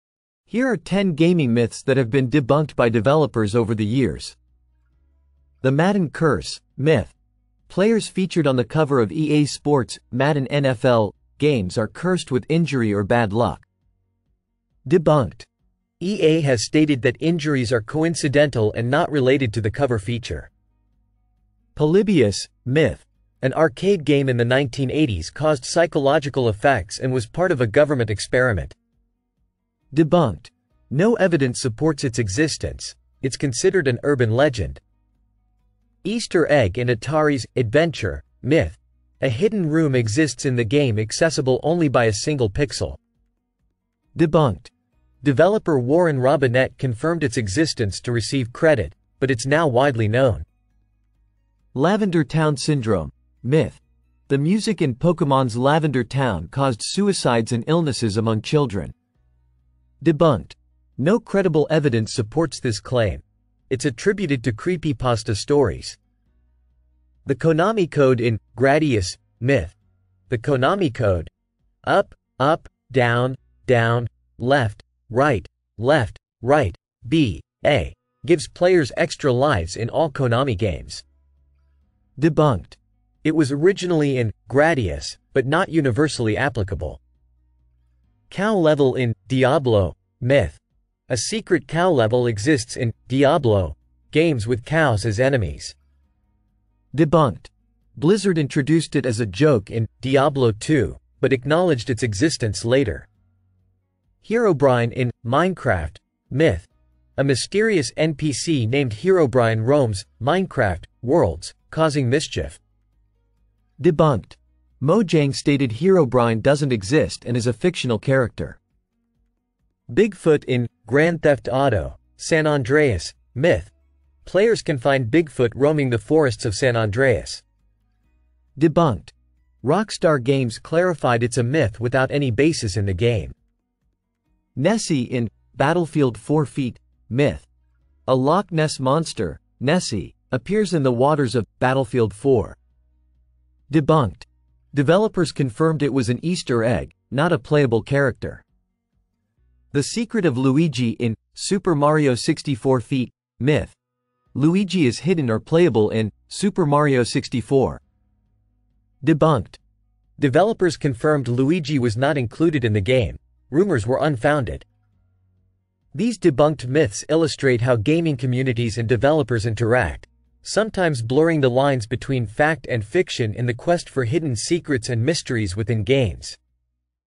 Top 10 Gaming Myths Debunked by Developers
Uncover the truth behind the top 10 gaming myths that developers have debunked! From urban legends to misconceptions, learn the real facts about gaming history and technology.